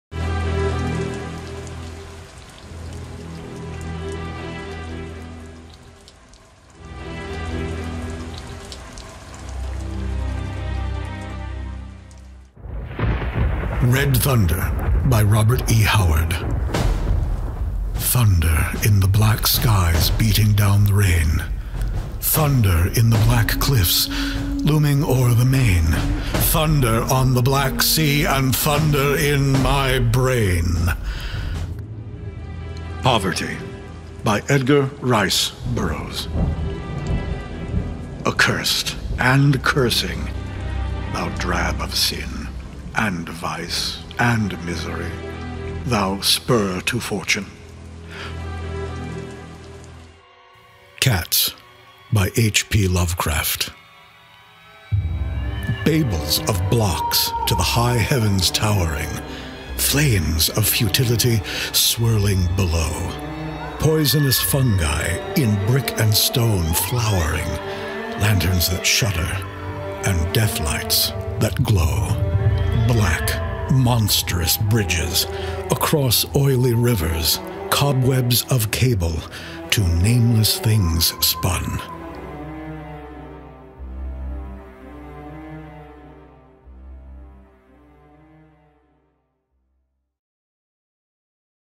SONGS OF GIANTS AUDIO BOOK PREVIEW 1
Readings by Mark Redfield. Music and Sound design by Jennifer Rouse. Based on the new illustrated book by Robert E. Howard, Edgar Rice Burroughs and H. P. Lovecraft that features the Poetry of Pulp. Available only through Kickstarter.